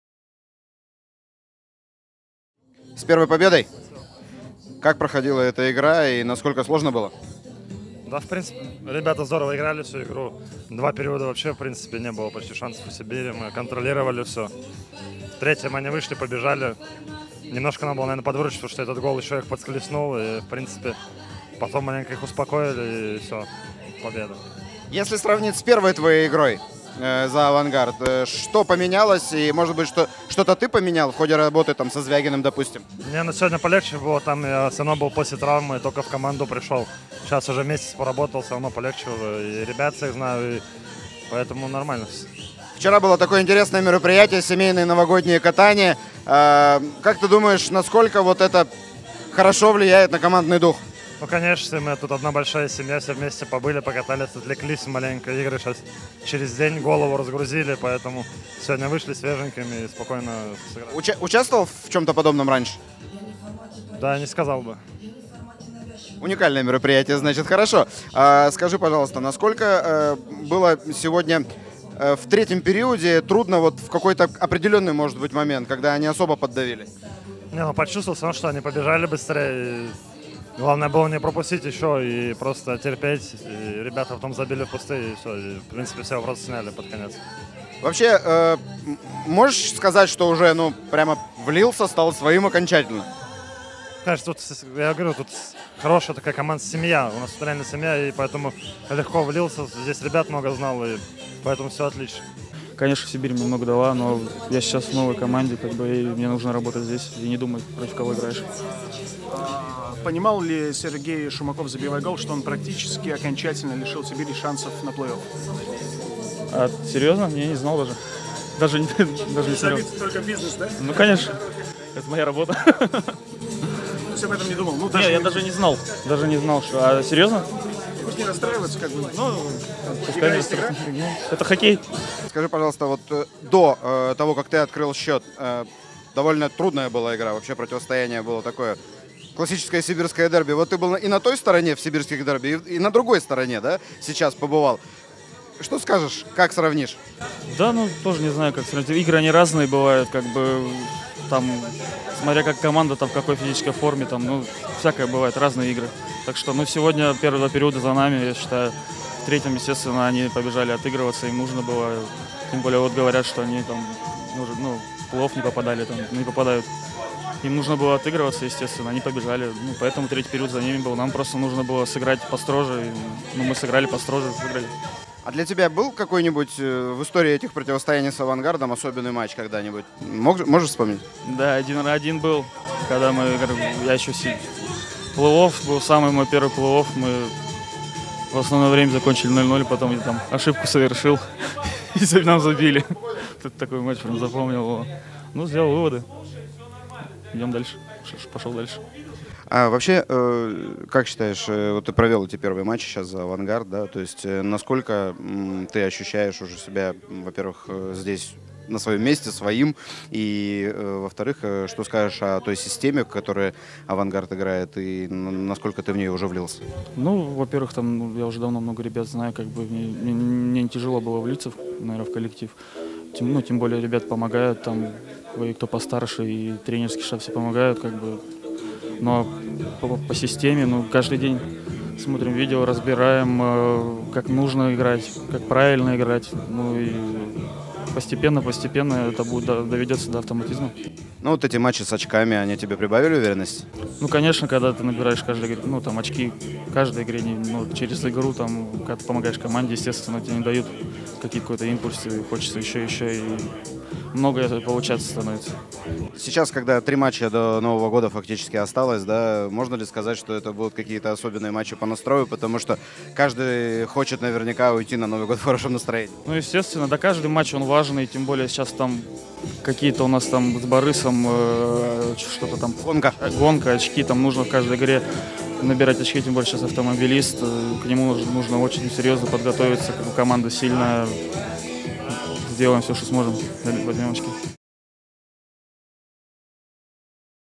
"Набираешь очки - получаешь импульс!" Налимов и Шумаков после победы над "Сибирью"
Вратарь Иван Налимов добился первой победы в составе "Авангарда" - и случилось это в матче с "Сибирью" (3:1). А нападающий Сергей Шумаков открыл счет в этой игре, забросив своей бывшей команде.